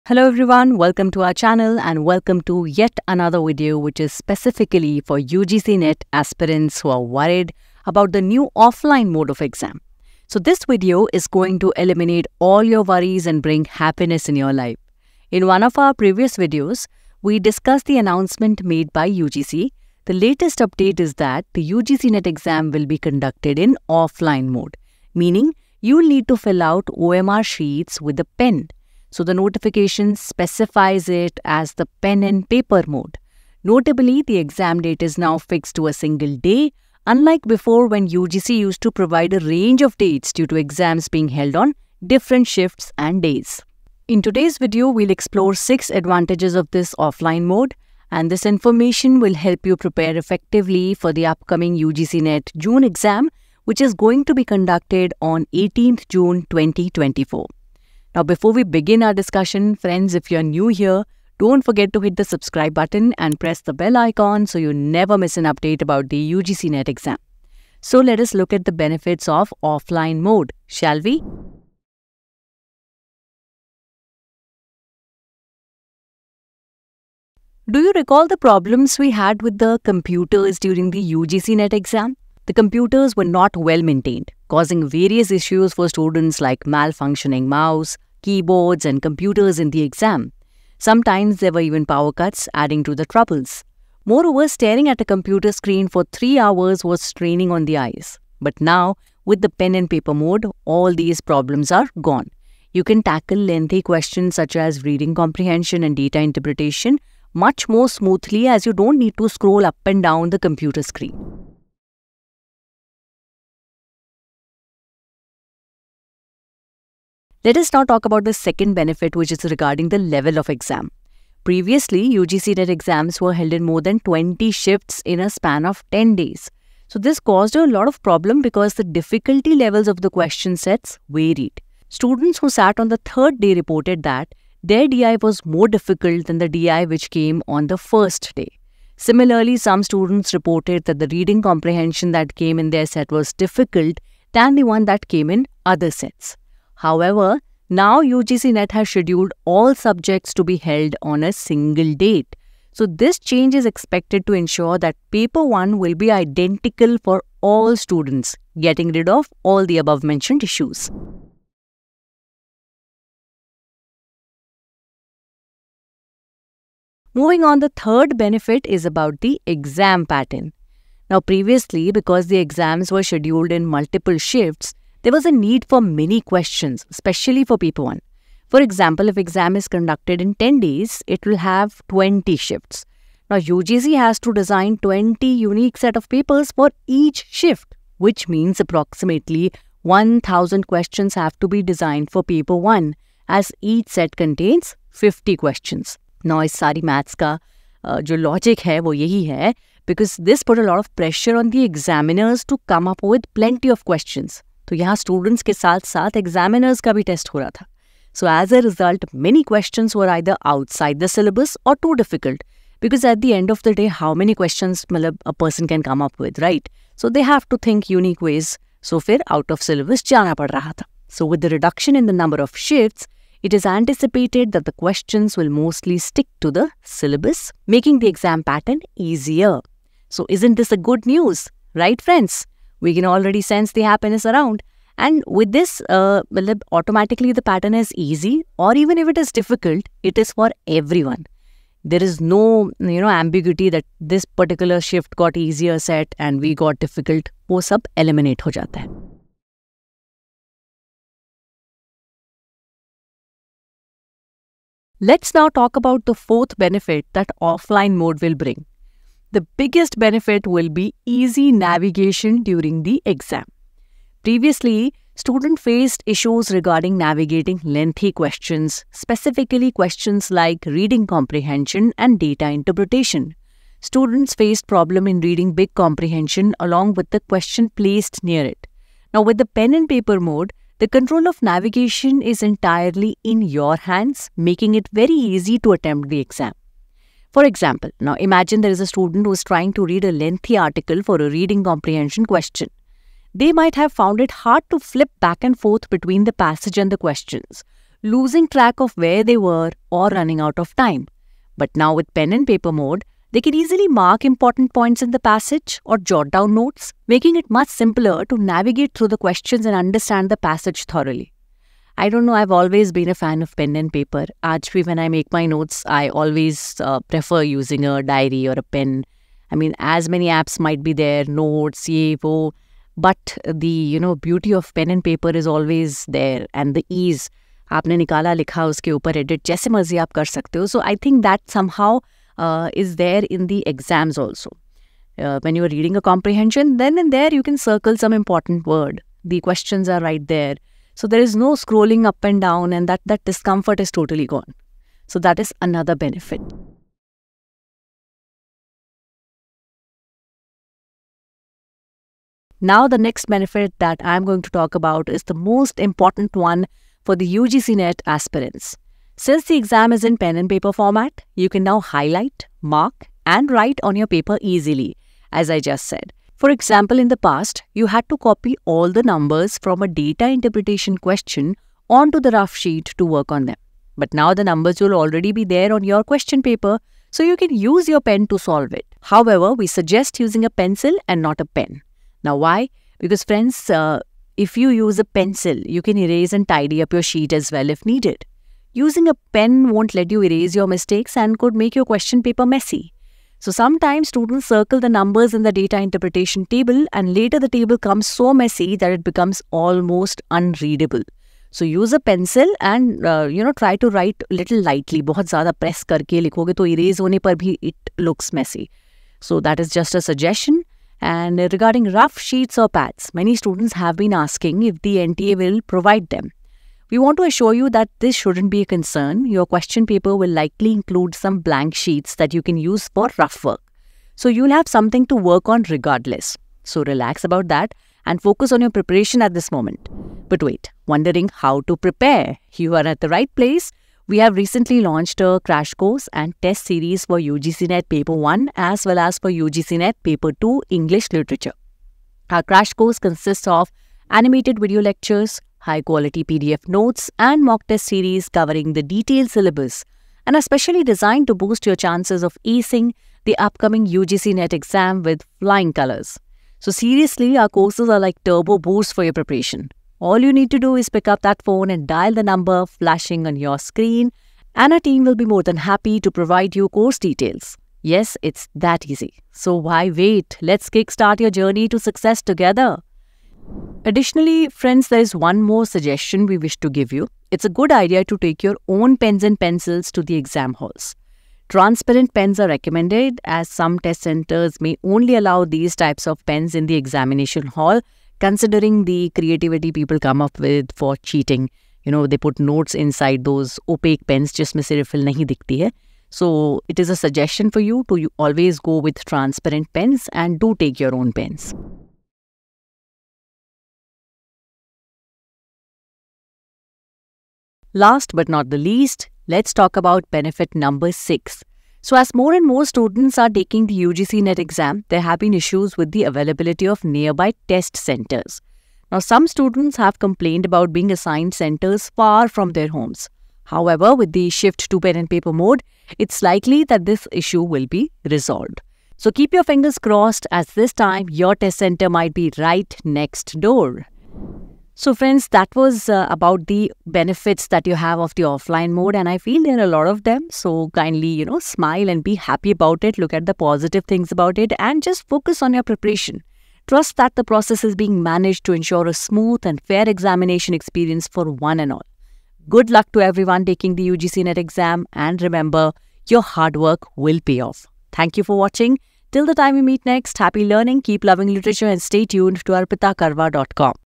Offline Mode can be a Huge ADVANTAGE for UGC NET Aspirants | Latest Exam Update
Are you gearing up for the upcoming UGC NET June 2024 exam? Wondering about the advantages of the offline mode? Look no further! In this video, we delve into why the UGC NET offline mode might just be the game-changer you need for your exam preparation.

With the latest updates and insights, we break down the benefits of opting for the offline mode and how it could significantly impact your performance. Stay informed and make the most out of your UGC NET journey.